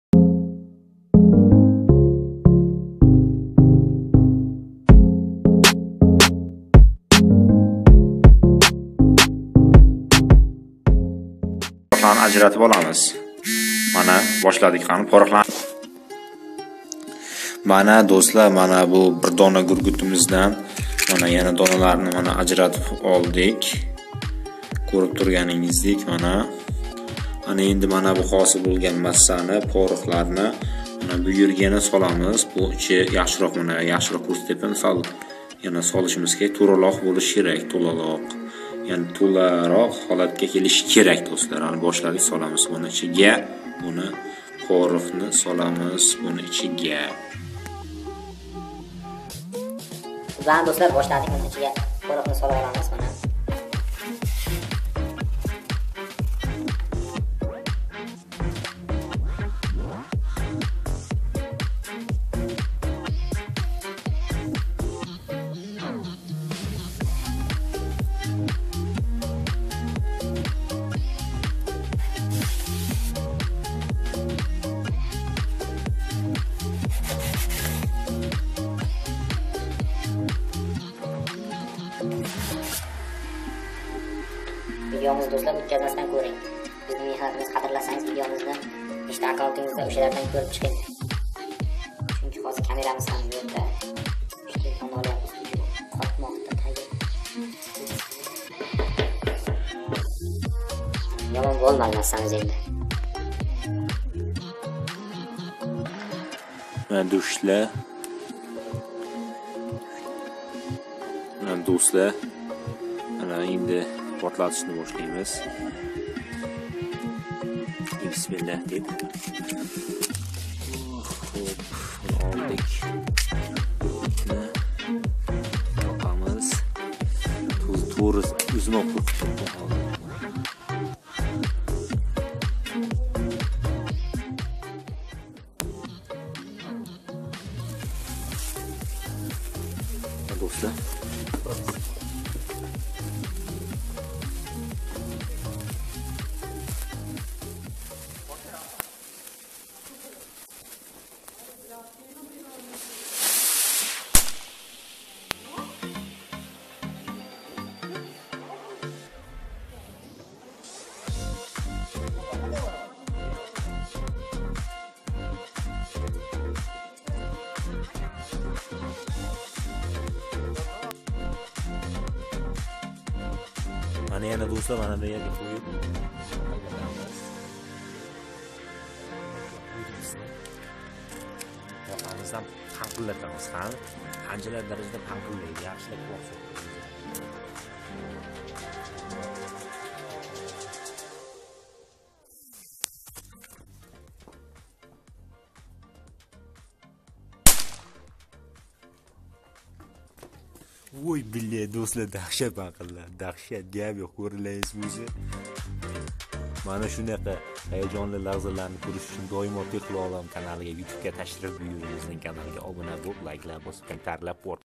Paqildoq tayyorlash 2-qism| Paqildoq yasash| Petarda tayyotlash
Paqildoq tayyorlash 2-qism| Paqildoq yasash| Petarda tayyotlash|Petarda yasash

Paqildoq tayyorlashning 2-qismi// Bu safar isboti koʻrsatilgan albatta koʻring xoziroq//
Sashil bot boom best new